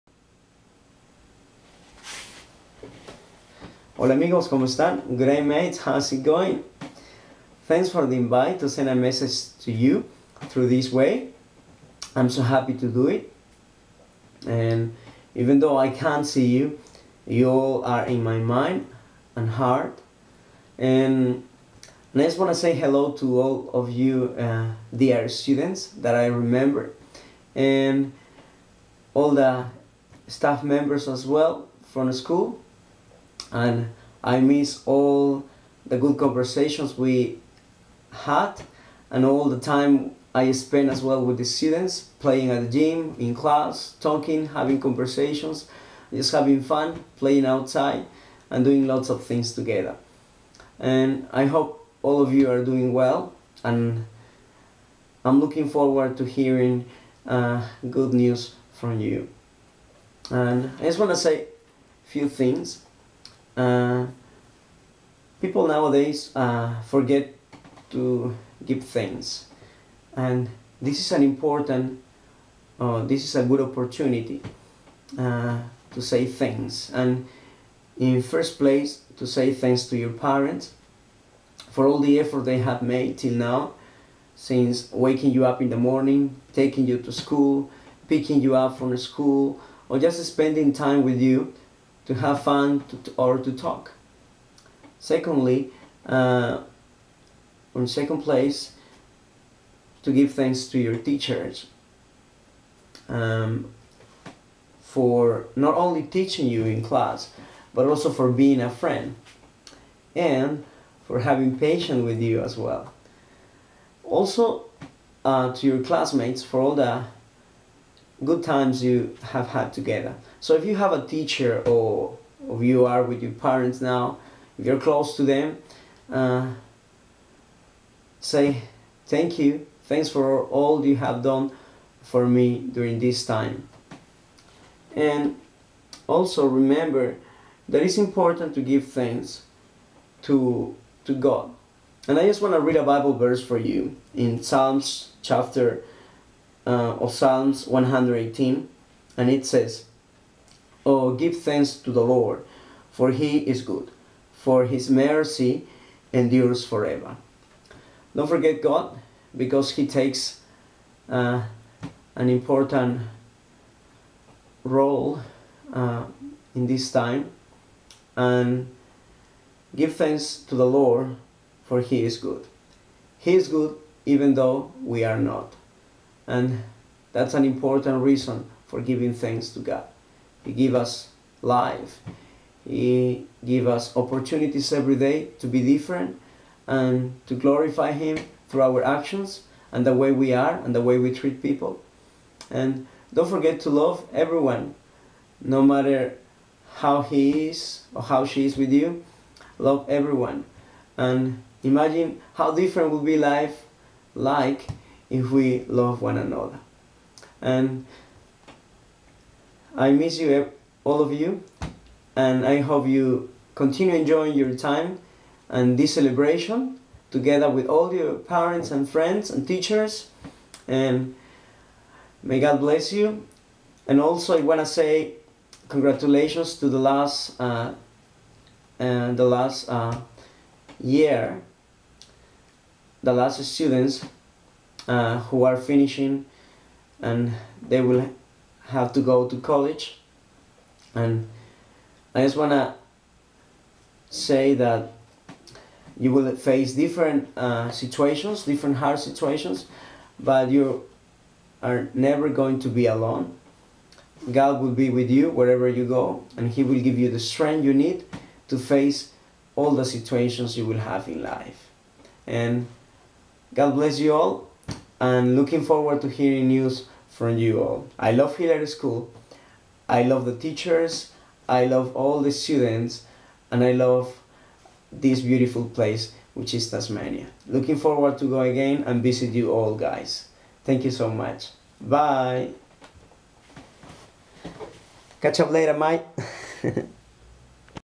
Greetings to all my mates at Hilliard Christian School - Tasmania
This video was recorded to send greetings to Students and Staff members at Hilliard Christian School.